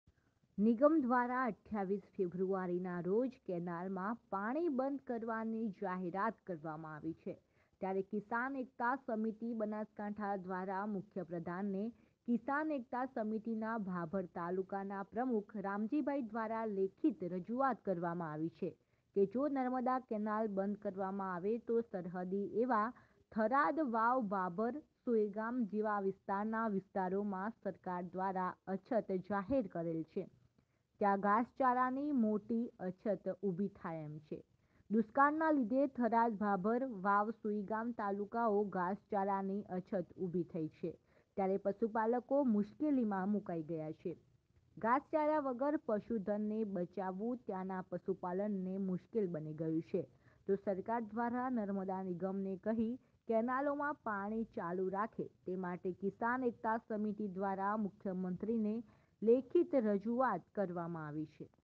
નમૅદા નિગમ દ્વારા 28 ફેબ્રુઆરી ના રોજ કેનાલમાં પાણી બંધ કરવા ની જાહેરાત કરવામાં આવી
નમૅદા નિગમ દ્વારા 28 ફેબ્રુઆરી ના રોજ કેનાલમાં પાણી બંધ કરવા ની જાહેરાત કરવામાં આવી છે ત્યારે કિસાન એકતા સમિતિ બનાસકાંઠા દ્વારા મુખ્ય પ્રધાન ને કિસાન એકતા સમિતિ ના ભાભર તાલુકાના પ્રમુખ રામજીભાઈ દ્વારા લેખિત રજુઆત કરવા માં આવી છે કે જો નમૅદા કેનાલ બંધ કરવામાં આવે તો સરહદી એવા થરાદ. વાવ.ભાભર. સુઇગામ જેવા વિસ્તાર ના વિસ્તારોમાં સરકાર દ્વારા અછત જાહેર કરેલ છે ત્યાં ઘાસચારા ની મોટી અછત ઊભી થાય એમ છે

દુષ્કાળ ના લીધે થરાદ ભાભર વાવ સૂઇગામ તાલુકાઓ ઘાસચારાની અછત ઉભી થઇ છે ત્યારે પશુપાલકો મુશ્કેલીમાં મુકાઈ ગયા છે  ઘાસચારા વગર પશુધનને બચાવવું ત્યાં ના પશુપાલન ને મુશ્કેલ બની ગયું છે તો સરકાર દ્વારા નમૅદા નિગમ ને કહી કેનાલો માં પાણી ચાલુ રાખે તે માટે કિસાન એકતા સમિતિ દ્વારા મુખ્યમંત્રી શ્રી ને લેખિત રજૂઆત કરવામાં આવી છે

જાહેરાત માટે સંપર્કઃ કિશોર તુવર - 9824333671/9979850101

banaskantha dist no.1 local news bulletin